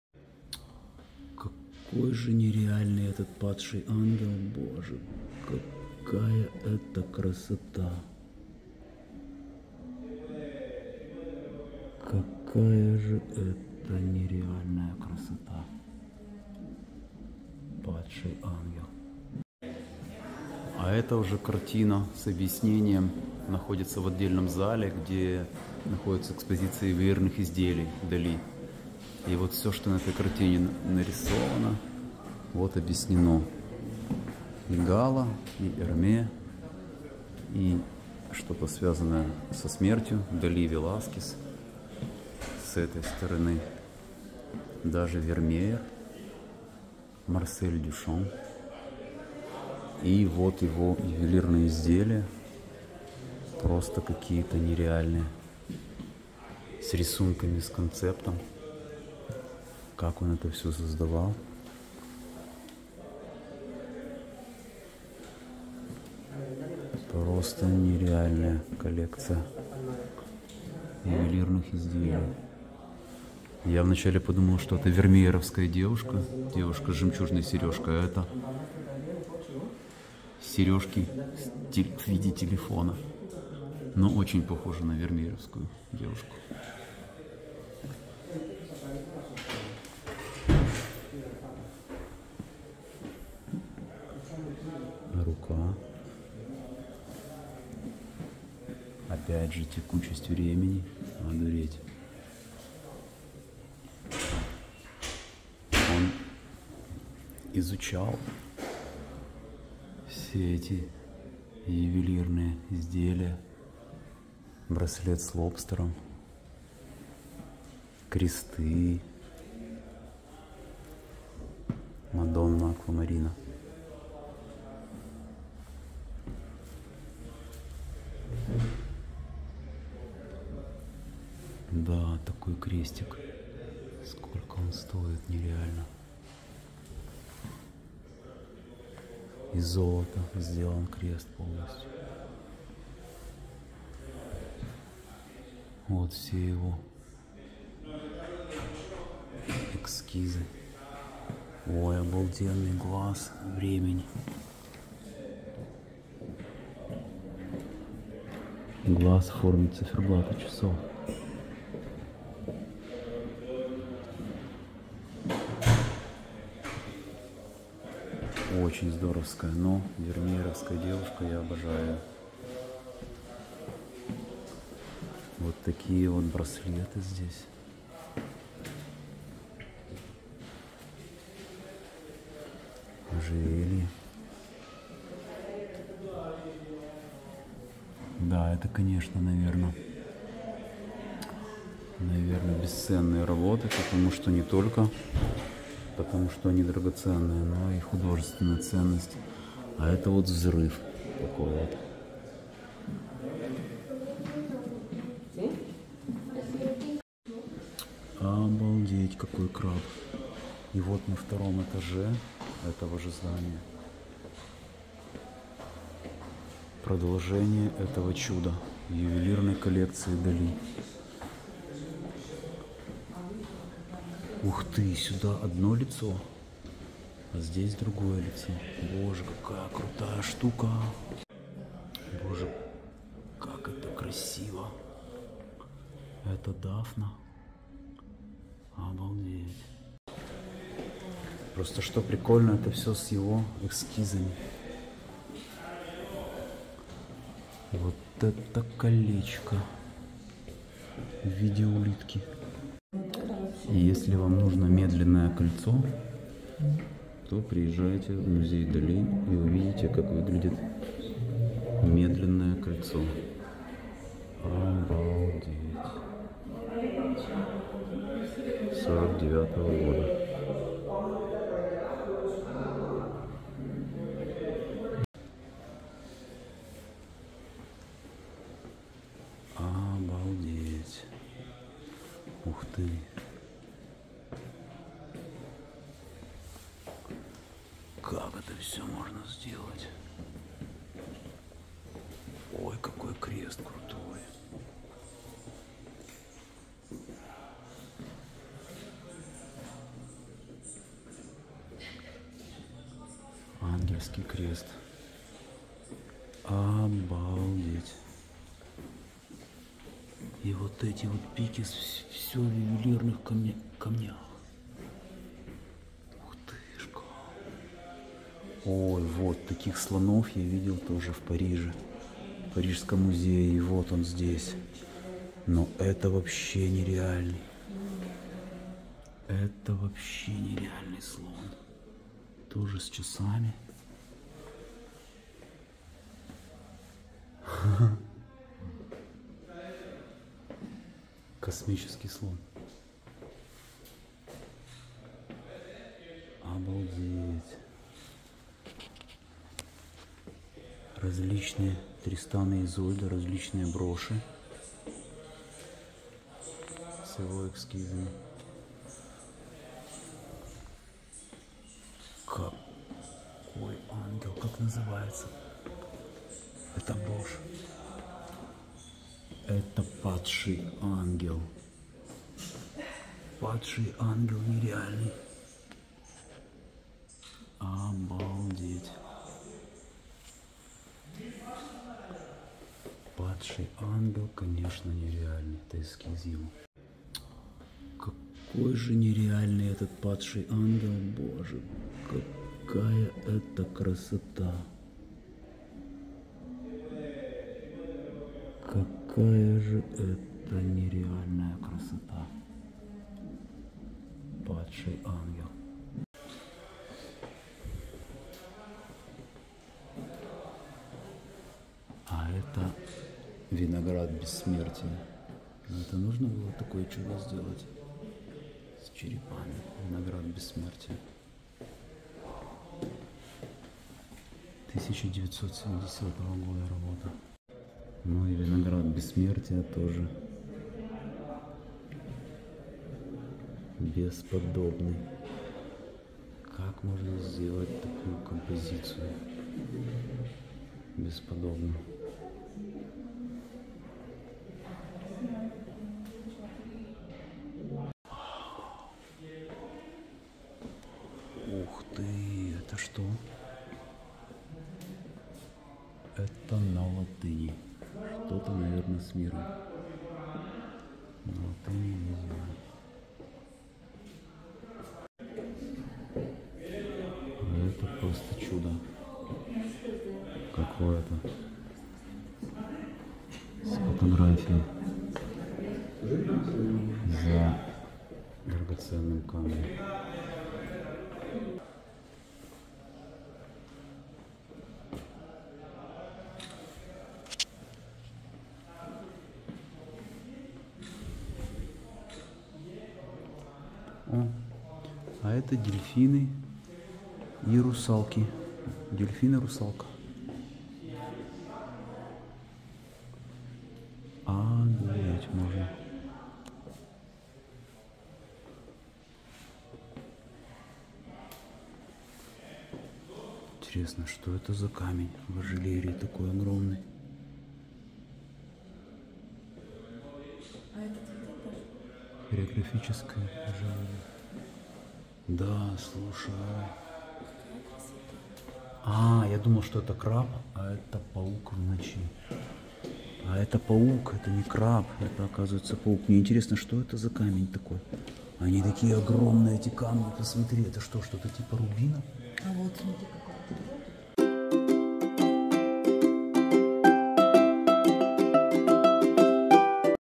Музей Дали, ЮВЕЛИРНЫЕ РАБОТЫ 4k⚜️dali
Театр-музей Дали — музей художника-сюрреалиста Сальвадора Дали, расположенный в городе Фигерасе, в Каталонии.

The Dalí Theater and Museum is a museum of the surrealist artist Salvador Dalí, located in the city of Figueres, in Catalonia.

samtaimzaifil